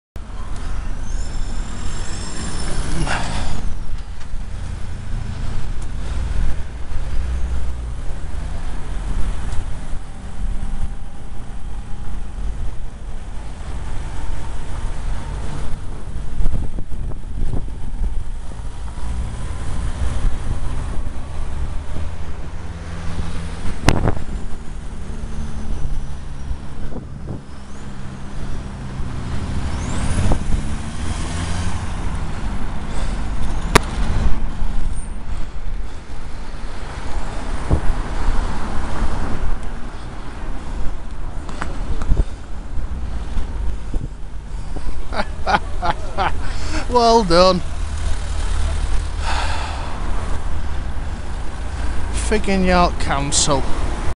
When not to use the cycle lane - Blossom Street
To be frank, this is just dire, expecially as they're resurfaced and replanned a lot of this section of road...